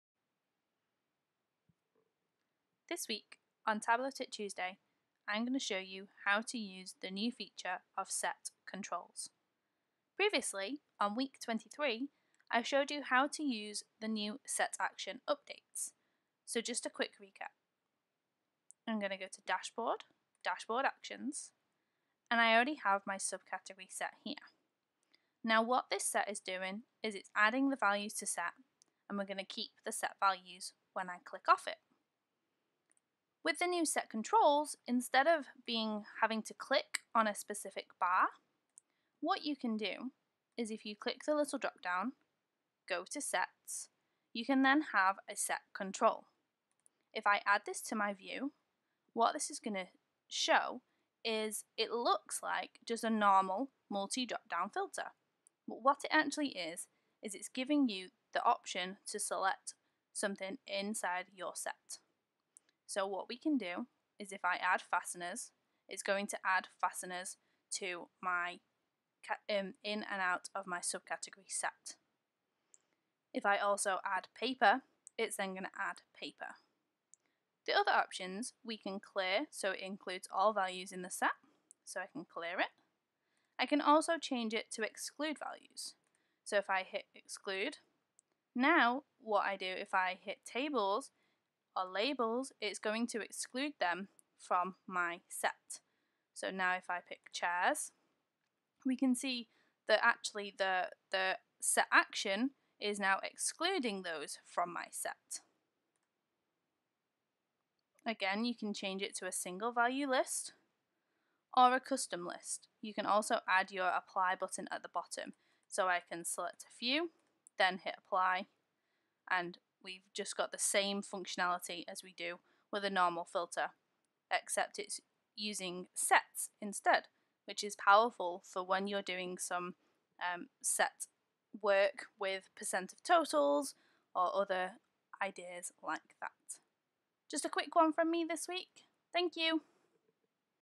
How to use Set Controls
Set controls give you, as the name says, more control over sets. You can add and remove items from a set with actions. In this video, Lorna shows you how it all works.